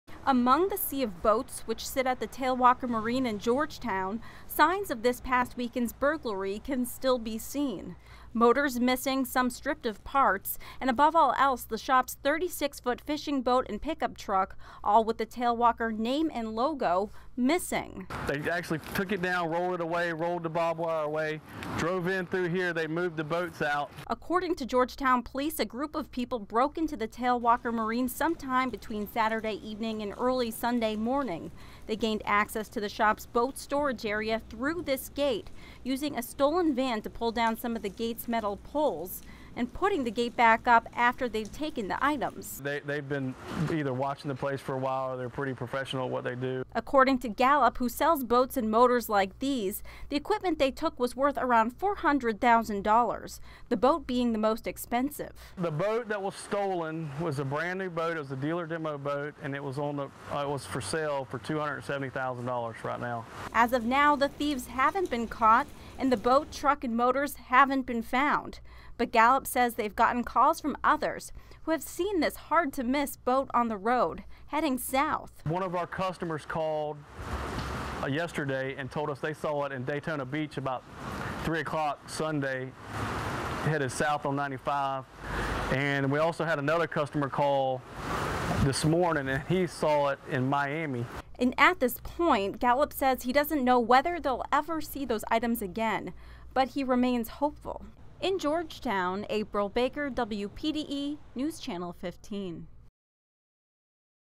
Thieves steal 36-foot boat, equipment from Georgetown business
On Tuesday, the Tailwalker Marine in Georgetown continued to search for those who stole nearly $400,000 worth of equipment this past weekend.